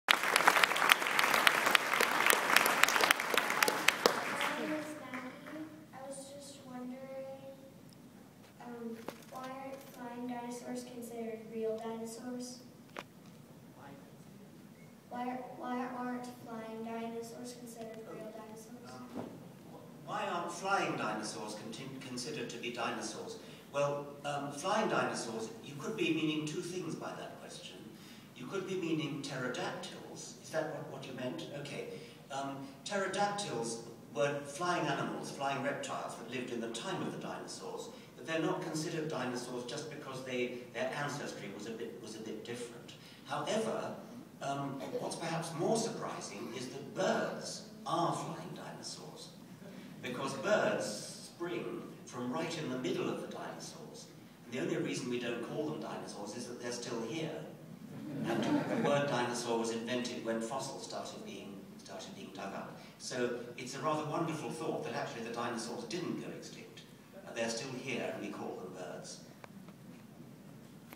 M asks Dawkins a question
M asks why flying "dinosaurs" aren't considered dinosaurs